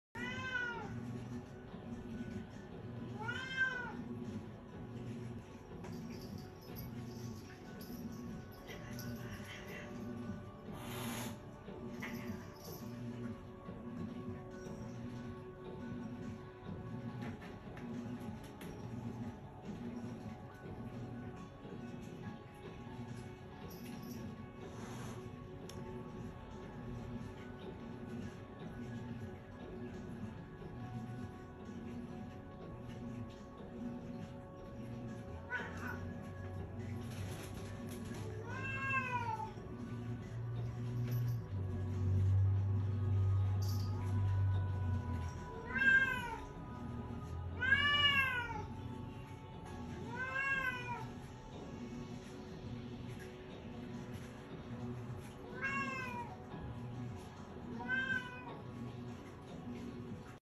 Cat Fighting
Cats Fighting and Meowing... These Two are Bloody Brothers , Cats Viral Video